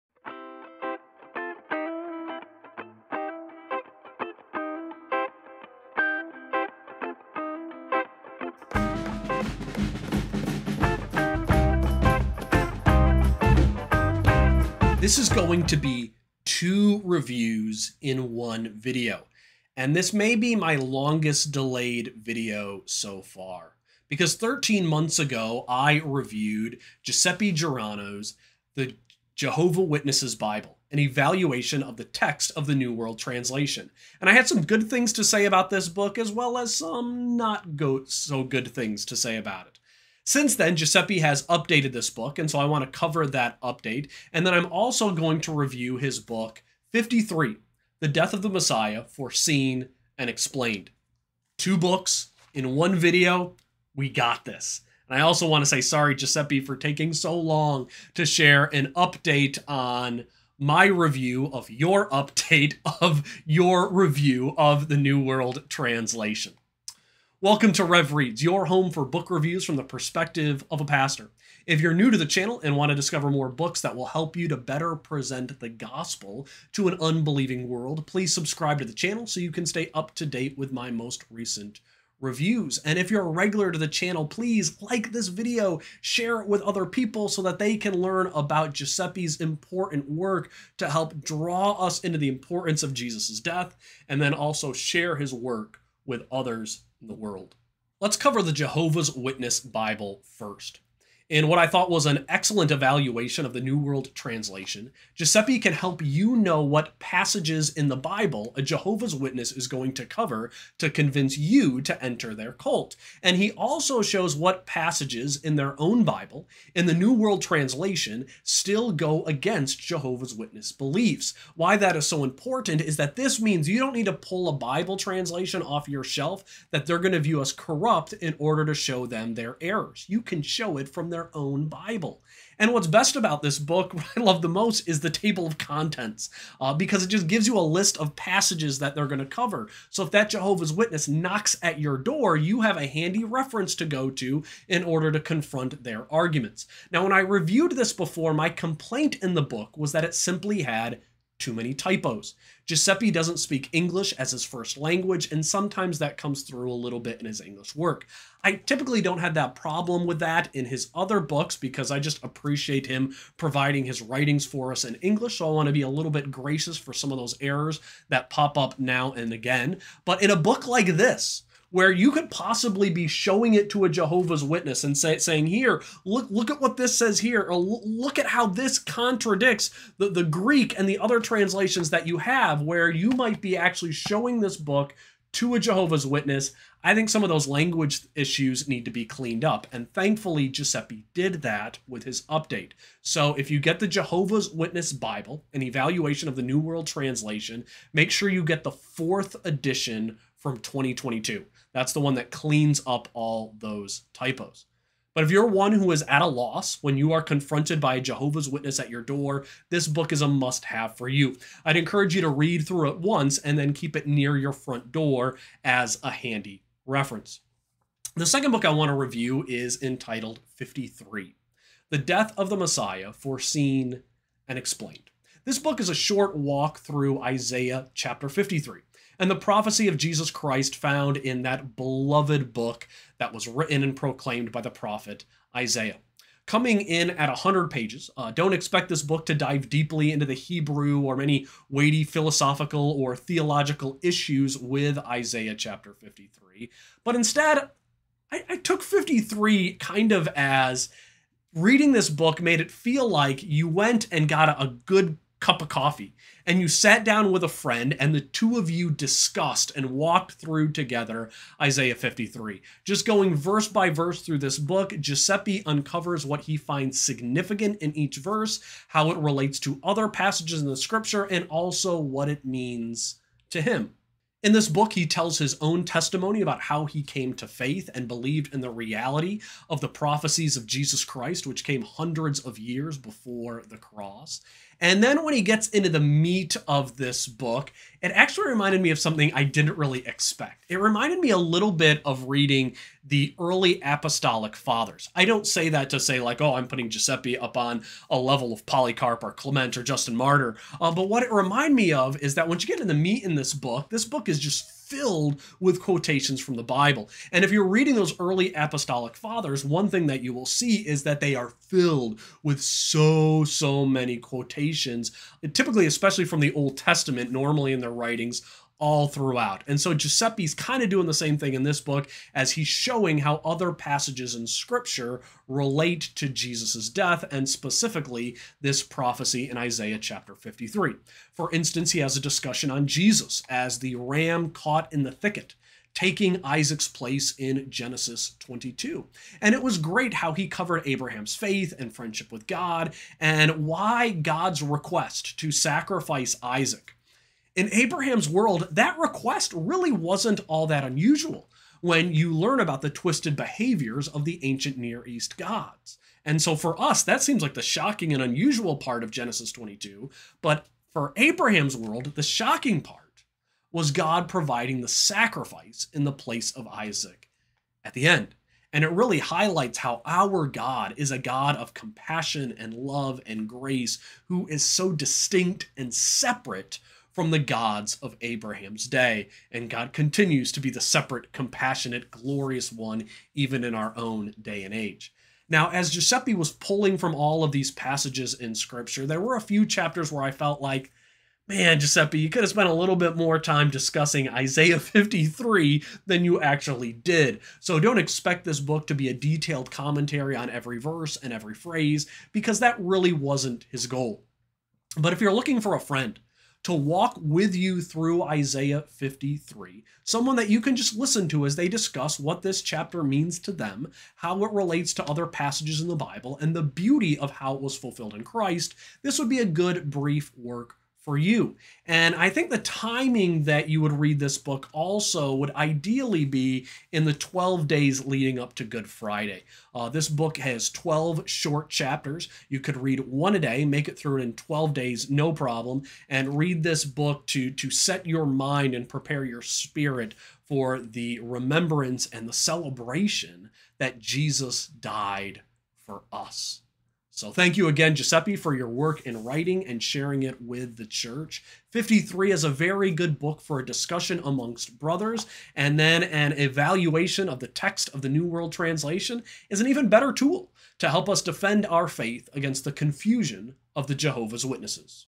53 and The Jehovah's Witness Bible: A Book Review
Support Rev Reads by becoming a member and be entered in our regular member book drawings.

Shawn reviews 53 and The Jehovah's Witnesses Bible by Giuseppe Guarino. He covers how Giuseppe made good upgrades on his critique of the New World Translation in the 4th edition of 2022. 53 is also an enjoyable walk with a friend through Isaiah 53.